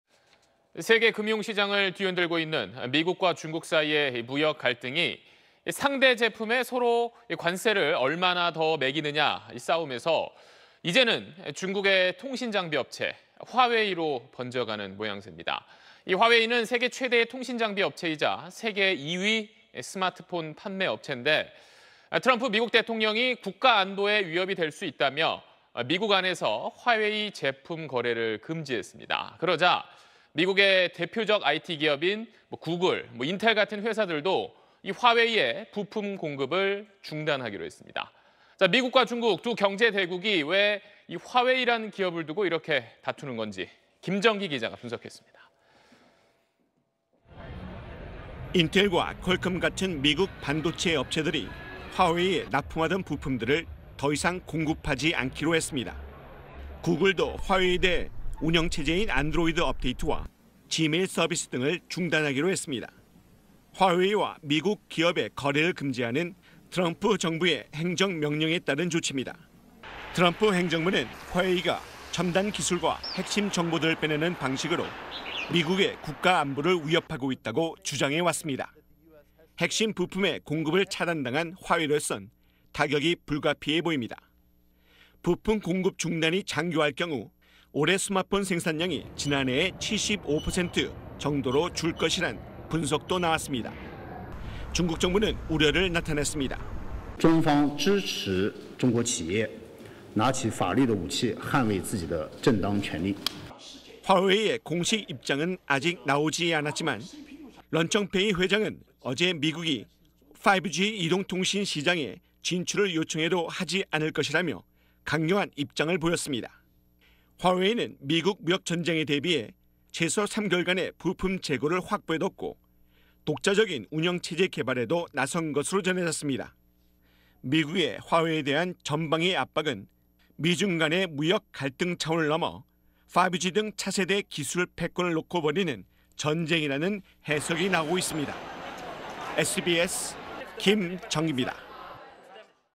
구글·인텔도 "화웨이와 거래 중단"…전방위 압박 / SBS
세계 금융 시장을 뒤흔들고 있는 미국과 중국 사이의 무역 갈등이 상대 제품에 서로 관세를 얼마나 더 매기느냐는 싸움에서 이제는 중국의 통신장비 업체 화웨이로 번져가는 모양새입니다. 화웨이는 세계 최대 통신 장비 업체이자 세계 2위 스마트폰 판매 업체인데 트럼프 미국 대통령이 국가 안보에 위협이 될 수 있다며 미국 안에서 화웨이 제품 거래를 금지했습니다. 그러자 미국의 대표적 IT 기업인 구글과 인텔 같은 회사들도 화웨이에 부품 공급을 중단하기로 했습니다.
미국과 중국, 두 경제 대국이 왜 화웨이라는 기업을 두고 이렇게 다투는 것인지 김정기 기자가 분석했습니다.